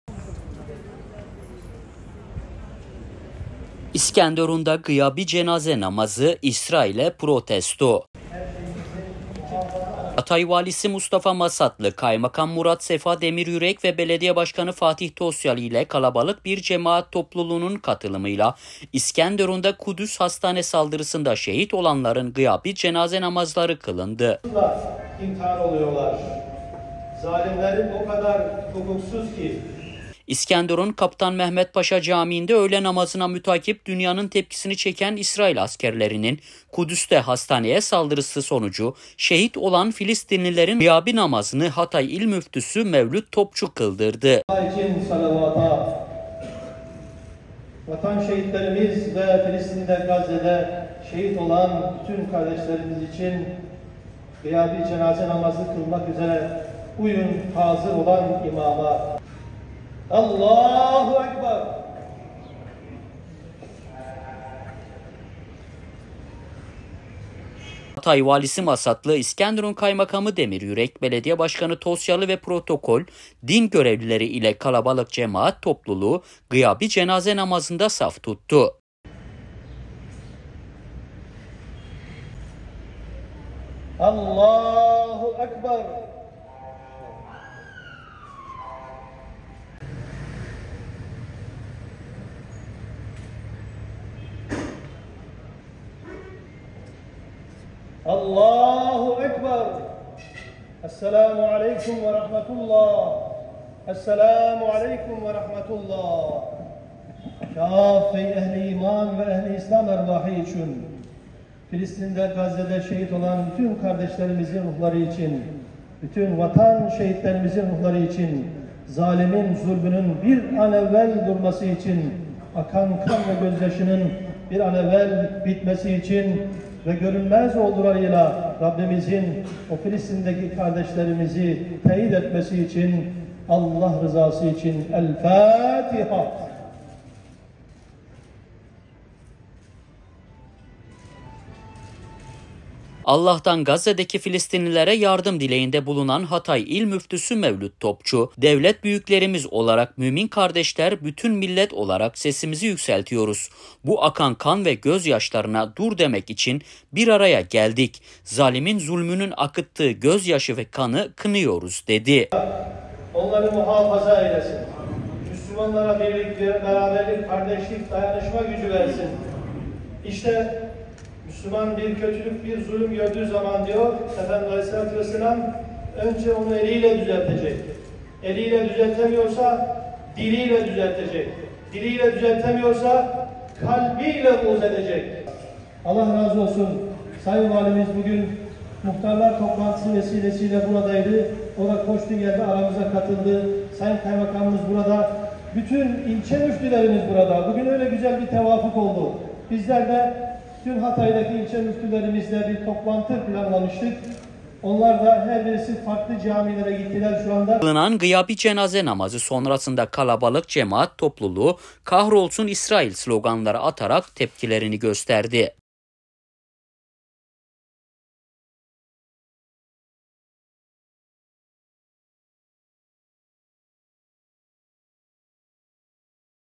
HATAY VALİSİ MUSTAFA MASATLI GIYABİ CENAZE NAMAZI KILDI
İskenderun’da gıyabi cenaze namazı, İsrail’e protesto!

Hatay Valisi Mustafa Masatlı, Kaymakam Murat Sefa Demiryürek ve Belediye Başkanı Fatih Tosyalı ile kalabalık bir cemaat topluluğunun katılımıyla İskenderun’da Kudüs Hastane saldırısında şehit olanların gıyabi namazı kılındı.